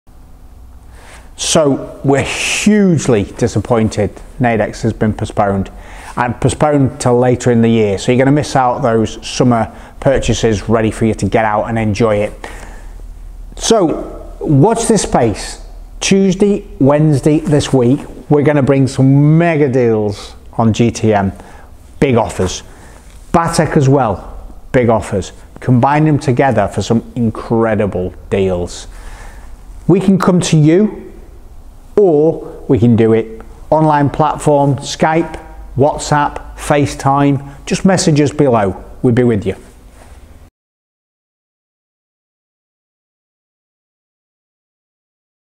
Cyclone Mobility Bring Naidex 2020 To You
We're bringing Naidex 2020 direct to you. After the disappointment of the postponement, we are bringing you our 2020 exhibition stand along with special offers that we had planned to share with you at the show.